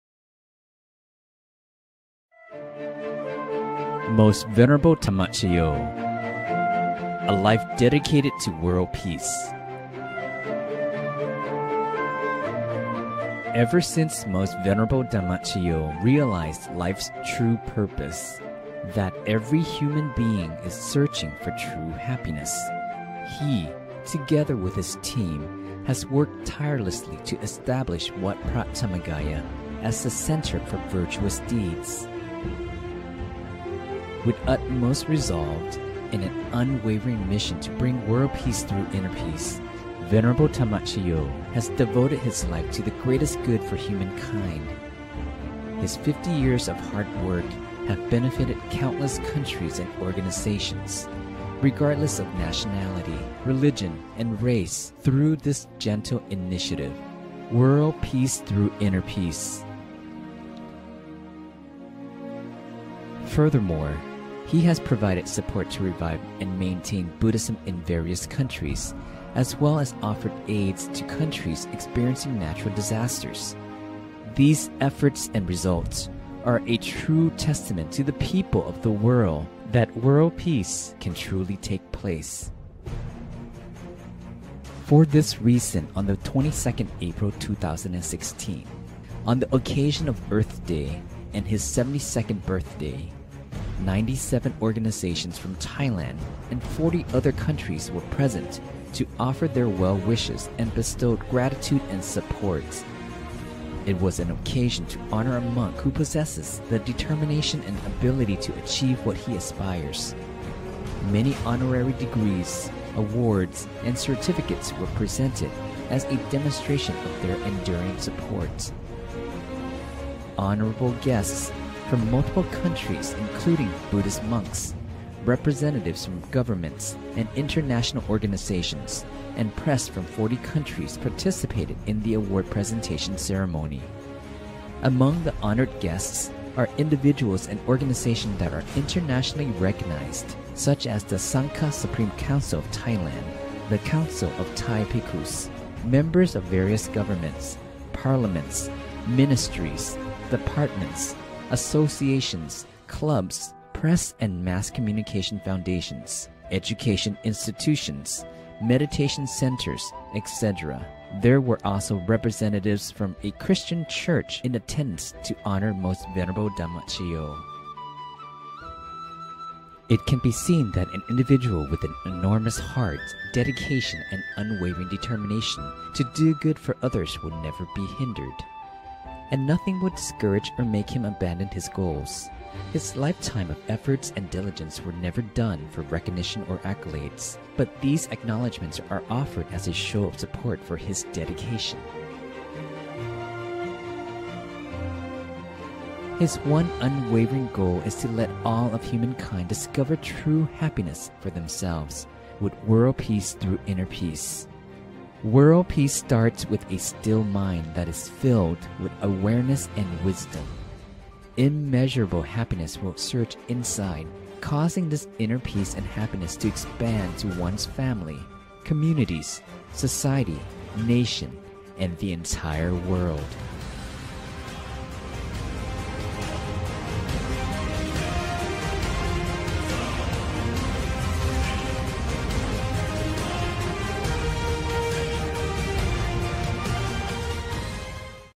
Most Venerable Dhammajayo: A life Dedicated to World Peace
"Creating a world filled with happiness through meditation is the noblest of all goals."

"The differences among beliefs, cultures, customs, nationalities, religions, or languages are only superficial external differences. Beneath these differences, there is a similarity."

"World Peace It can not happen through one person. We can not accomplish it alone. It comes from the thoughts of all beings. Excluding no one."

"The dream of world peace will never come true if the dreamer is unable to find his or her own inner contentment."

“Purify the mind thru meditation leads to greater happiness, kindness, selflessness and love for all beings.”

The most Venerable Dhammajayo Bhikkhu